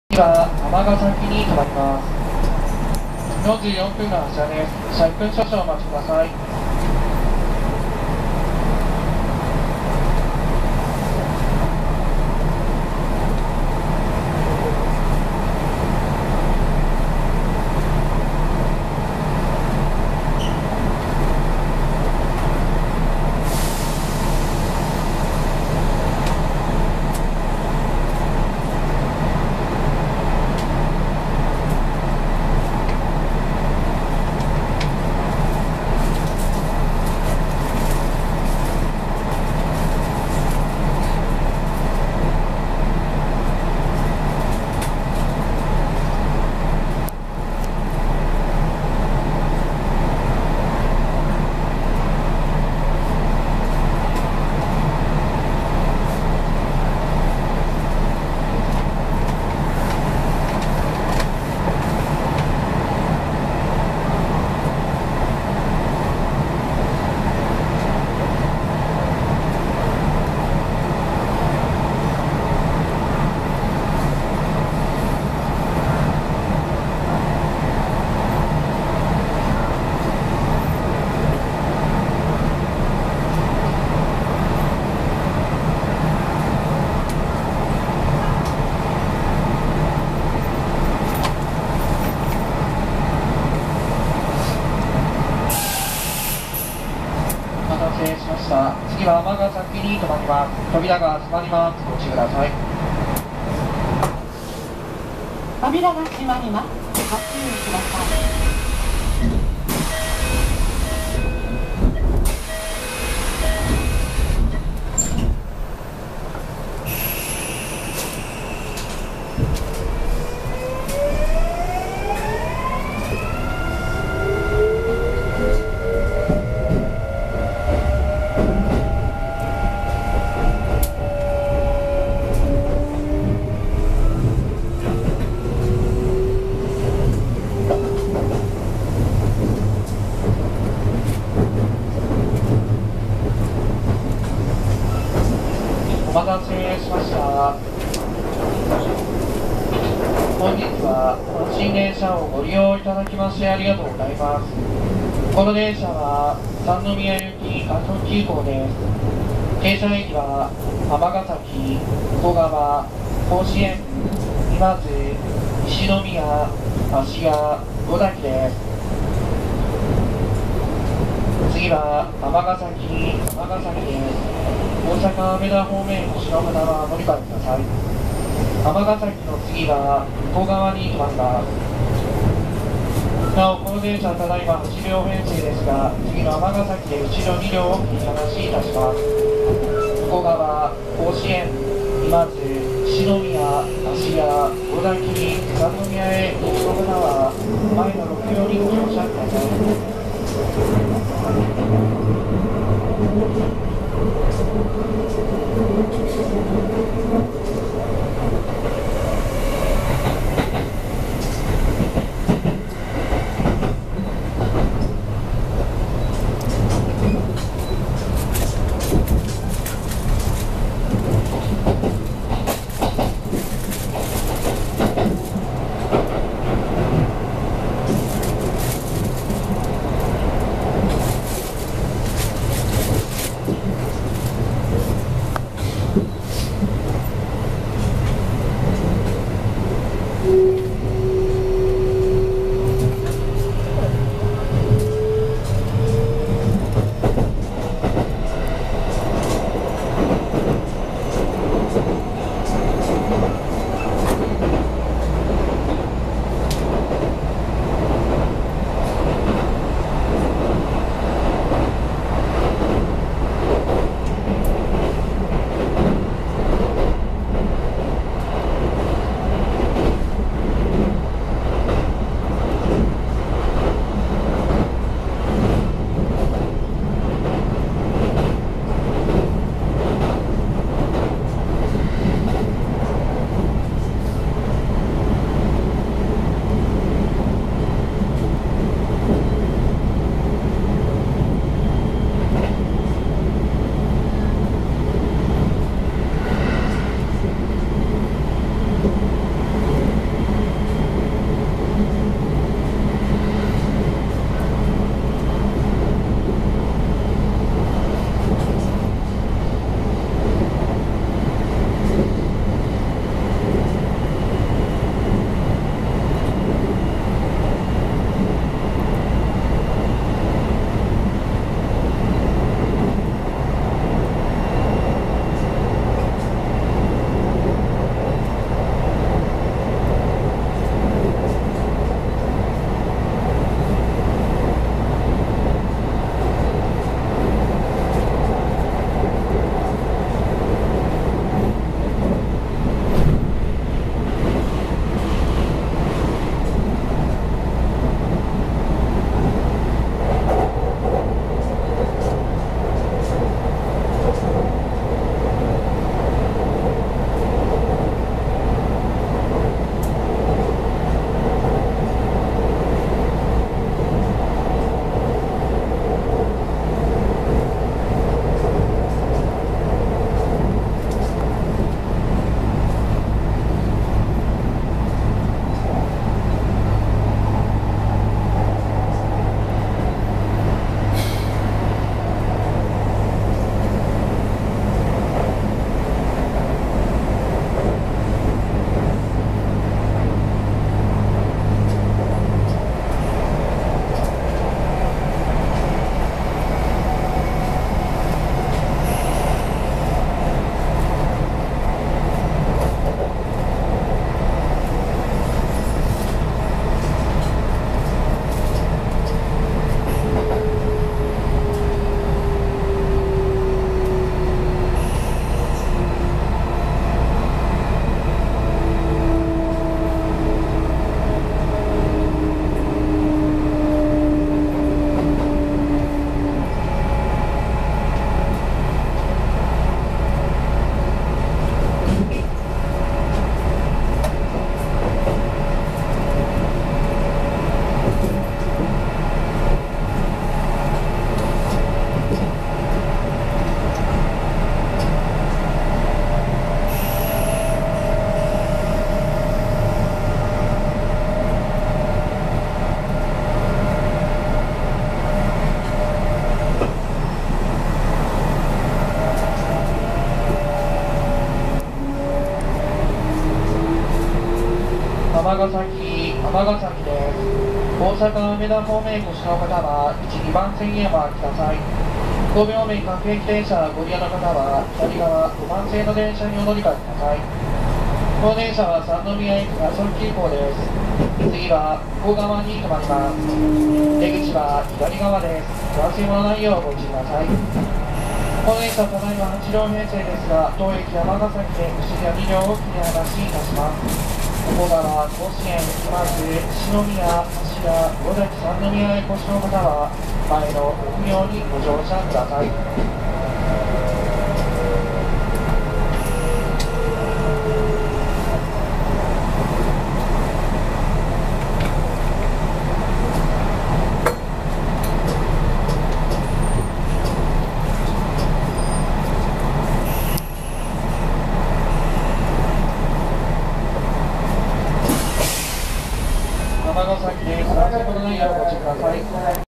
【近畿日本鉄道】5820系DH22 側面展望 阪神三宮行き快速急行 西九条⇒尼崎
近鉄奈良線で活躍している5820系　（シリーズ21）のDH22（5822F、三菱IGBT）　阪神なんば線西九条から尼崎までの快速急行の側面展望を撮影しました。IGBT素子VVVFですが実際GTO並みの爆音を出しますｗ